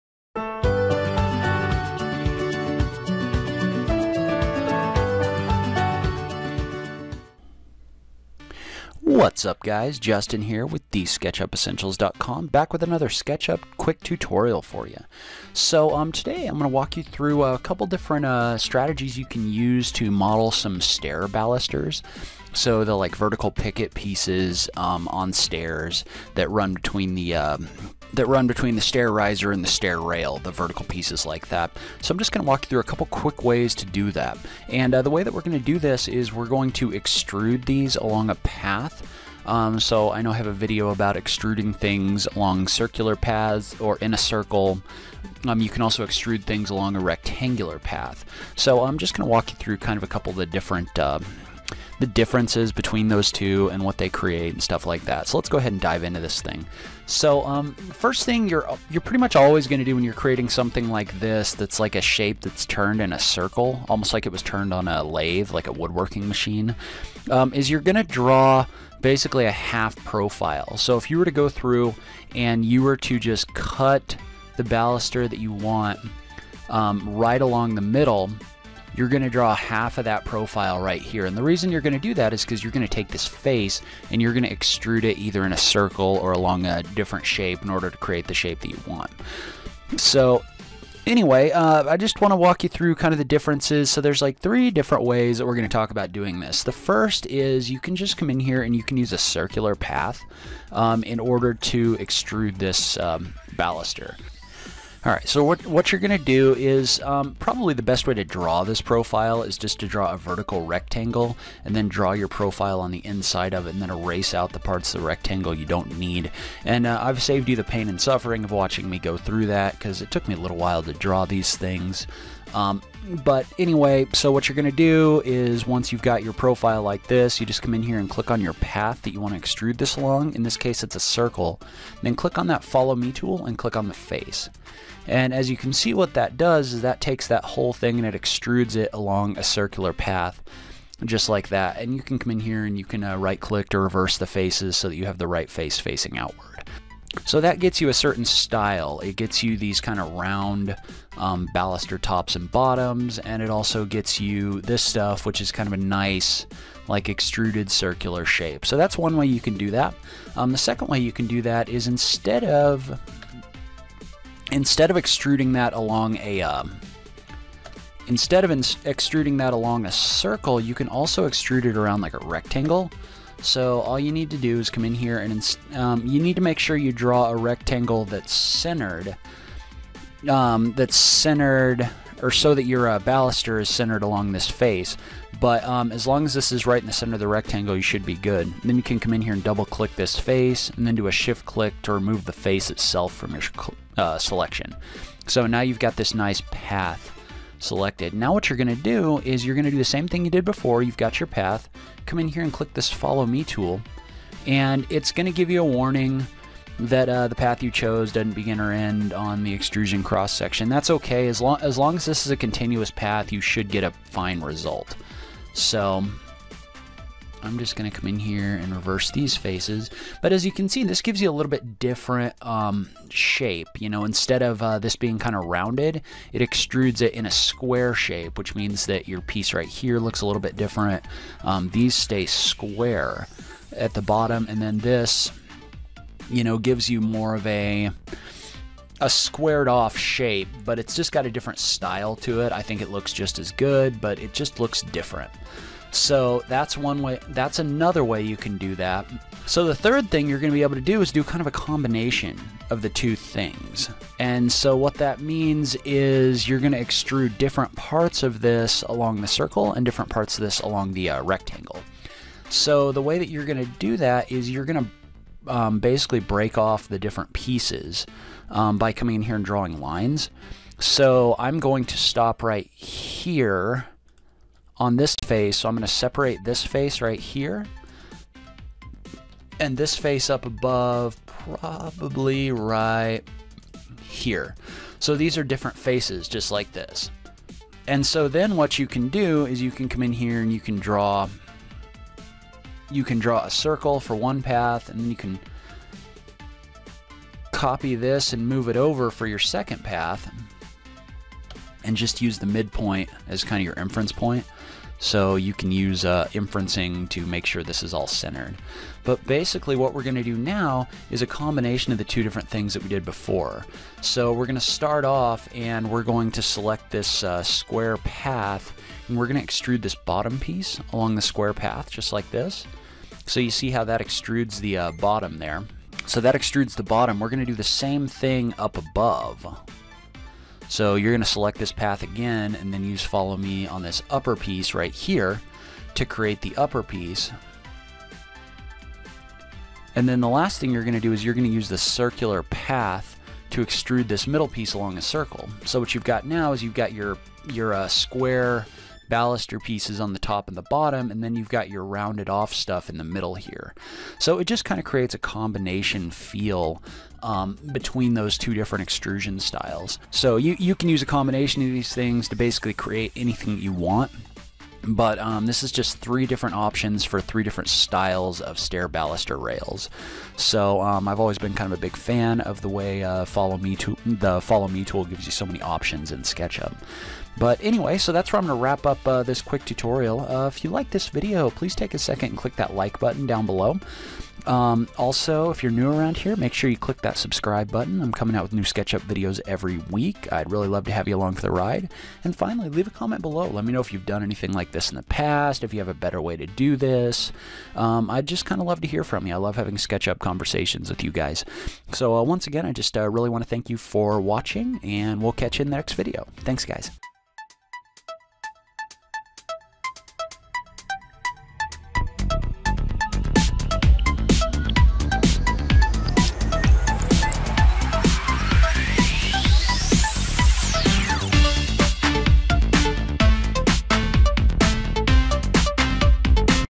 Creating Stair Balusters in SketchUp with the Follow Me Tool
This tutorial will teach you to use the follow me tool in SketchUp to create different types of stair balusters (pickets).

Want to support what I do? and consider purchasing more in depth SketchUp training.
This SketchUp tutorial will teach you to use the follow me tool to create various styles of stair balusters or pickets.
Start by drawing a profile view of the baluster you’d like to create (preferable on a vertical rectangle as a canvas.)
Once you’ve drawn your profile, draw a circle and/or a square to use as a path for the follow me tool.
Select your path, then click once on your face to extrude along your path to create your baluster.
You can also break your baluster up into different shapes to extrude them separately.